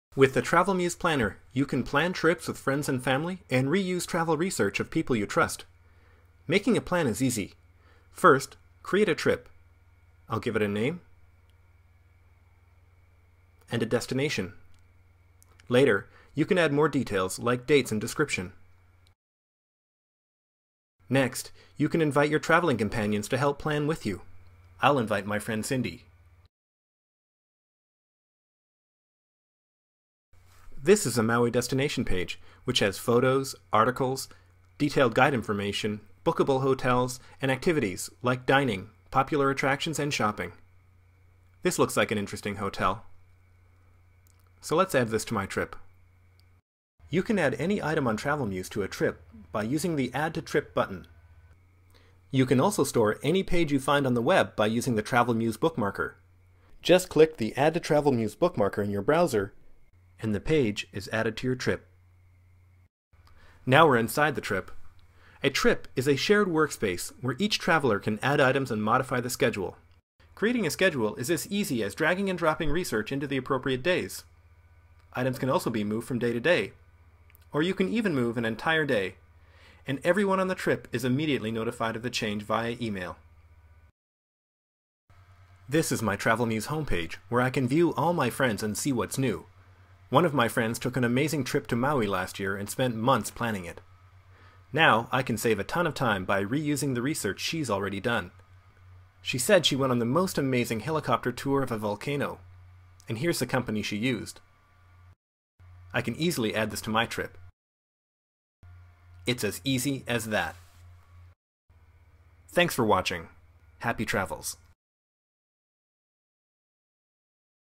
TravelMuse Unveils Revolutionary Social Trip Planner
Abandon the manila folders! TravelMuse Inc., creators of TravelMuse.com, the online destination for finding inspiration, planning trips and booking travel, today announced the launch of the TravelMuse Planner with social trip planning tools. The Planner is the first travel solution to harness social networking and enable co-travelers to connect and collaborate on trips. The Planner makes organizing trips a breeze by giving individuals and groups of travelers the ability to research, store and share travel information in one centralized location on the Web.